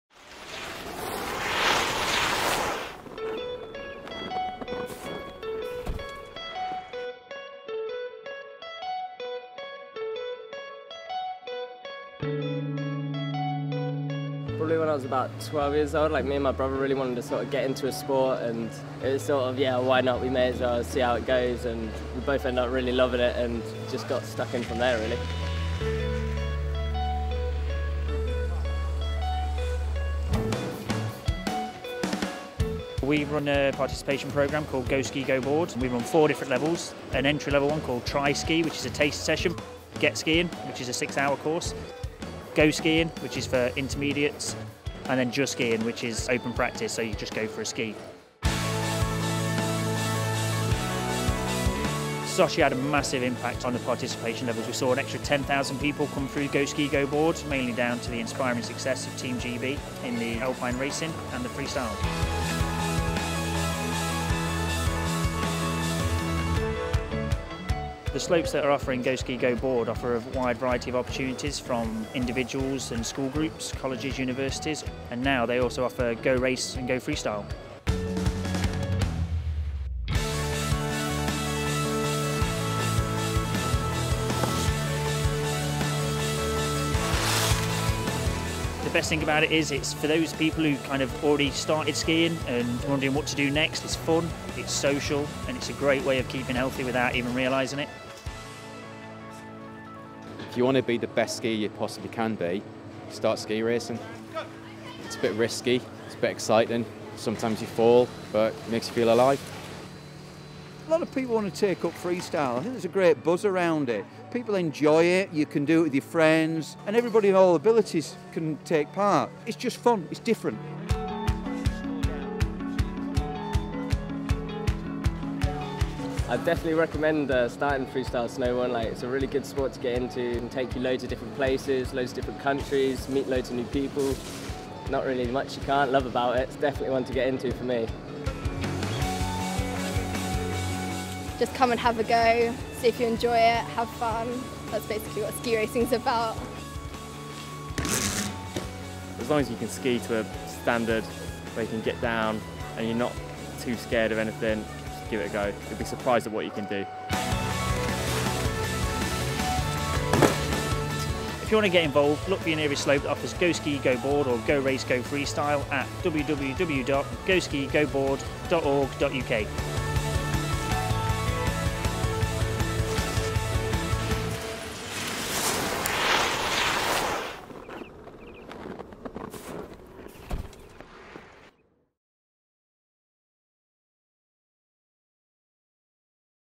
Learn more about GO SKI GO BOARD
Inspired to take up skiing or snowboarding?
Inspired to take up snowsports? With GO SKI GO BOARD you can learn to ski or snowboard right here in the UK, or get involved in GO RACE GO FREESTYLE.

This video tells you all about it and how to get involved...

to find your nearest activity and sign up for free to receive notifications of the latest slope or club offers.